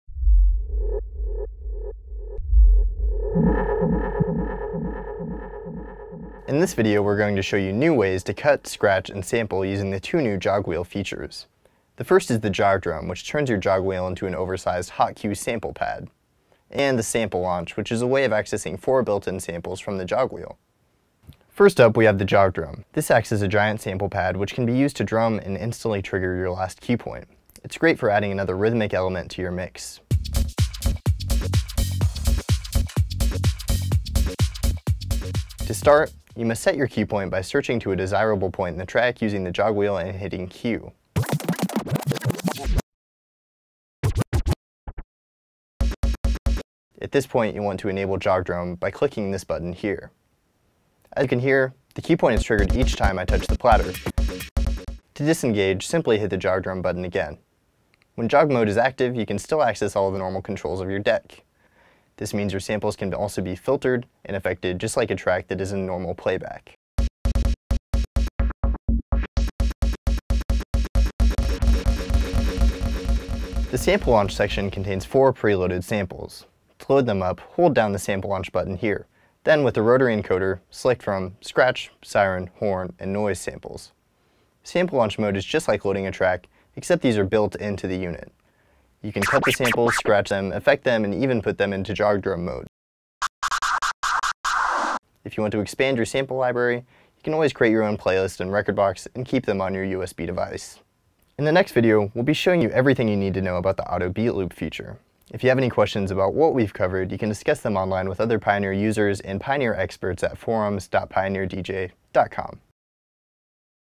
XDJ-Aero Jog Drum and Sample Launch Tutorial
In this video we are going to show you new ways to cut, scratch and sample using the two new jog wheel features.

00:22  Jog Drum
01:13  Sample Launch

If you have any questions about what we've covered, you can discuss them online with other Pioneer users and Pioneer experts @

Please fill out our quick survey to help us improve our tutorial videos